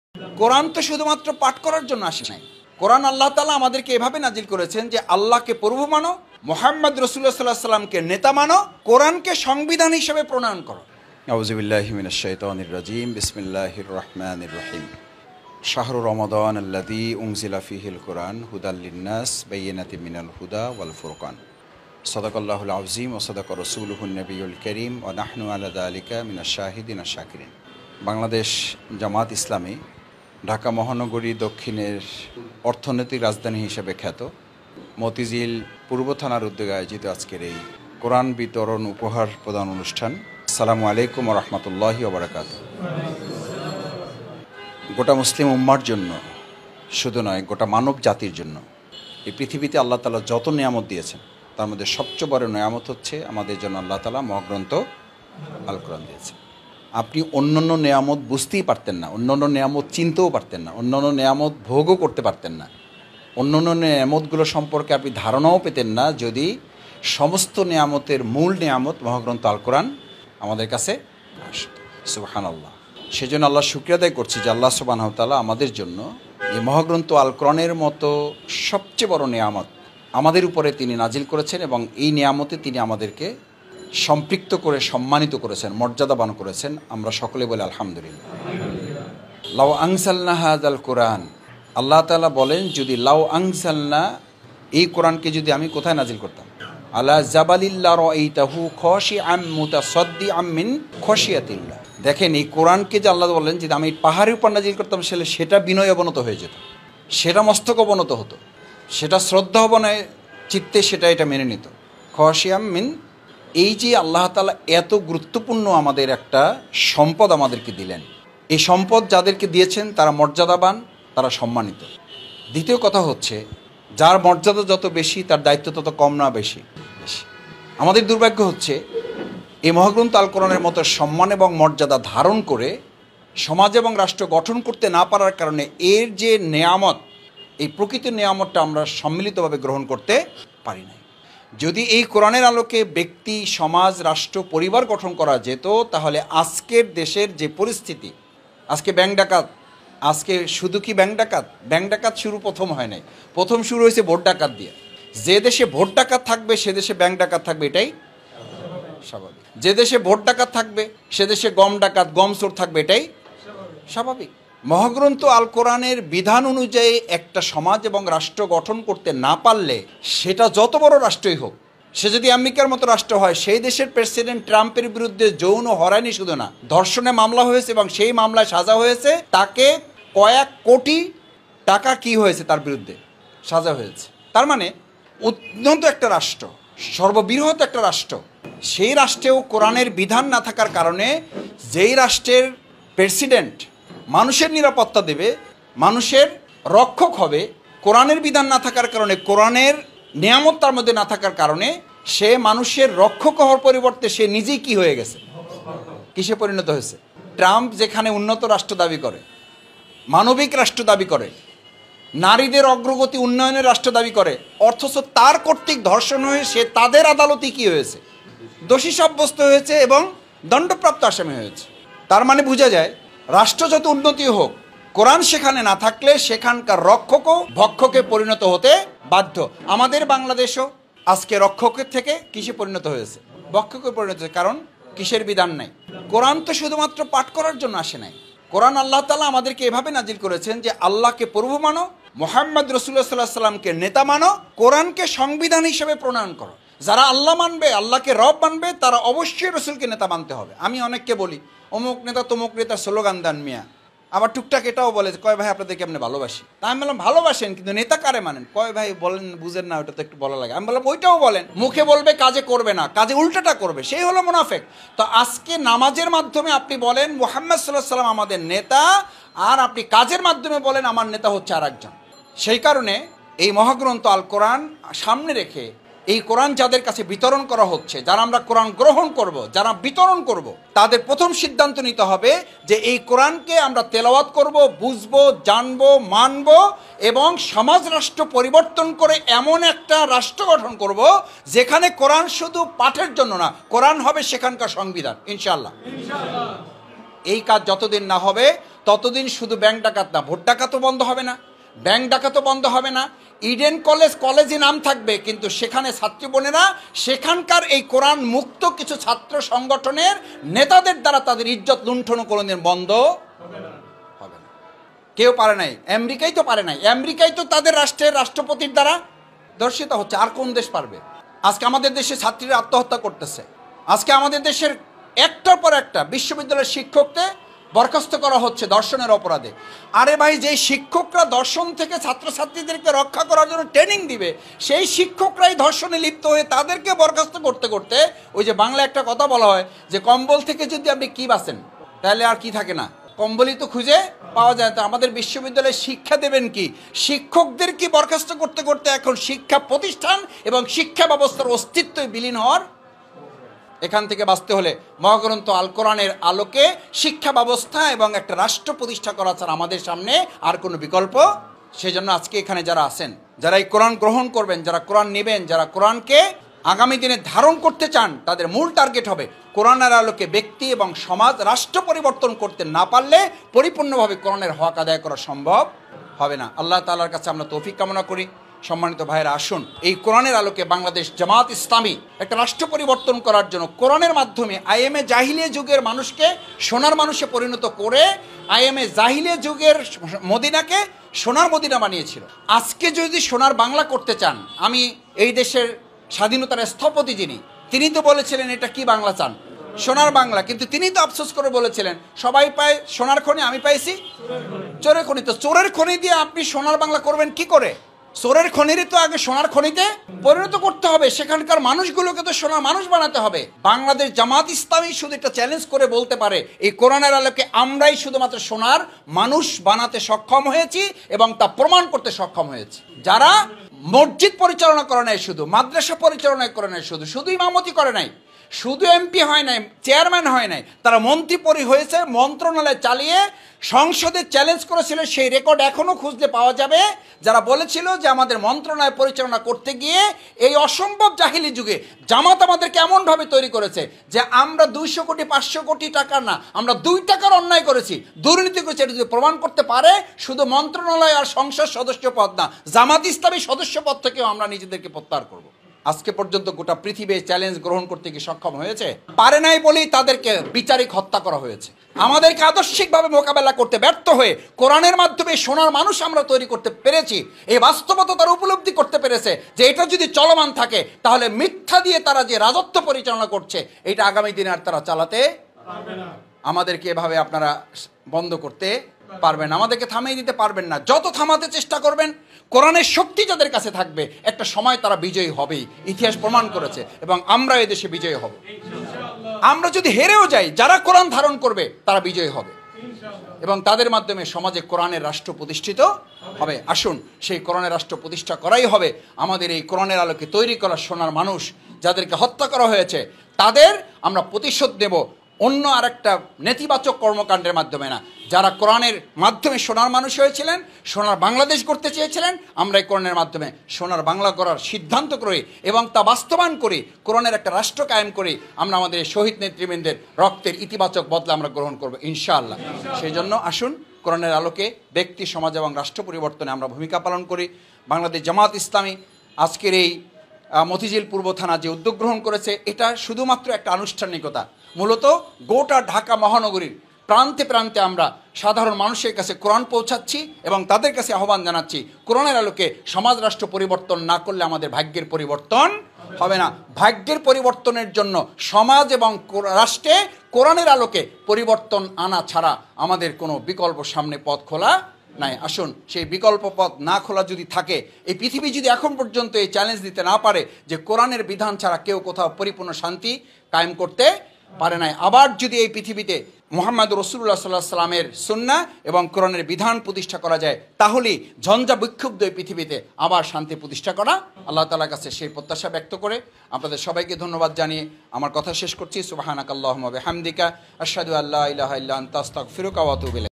দেশে যেসব অন্যায়, জুলুম, অনাচার ও লুটপাট চলছে কুরআনের বিধান প্রতিষ্ঠা ছাড়া তা বন্ধ হবে না
000   দেশে যেসব অন্যায়, জুলুম, অনাচার ও লুটপাট চলছে কুরআনের বিধান প্রতিষ্ঠা ছাড়া তা বন্ধ হবে না।

- কুরআন উপহার প্রদান অনুষ্ঠানে ড. শফিকুল ইসলাম মাসুদ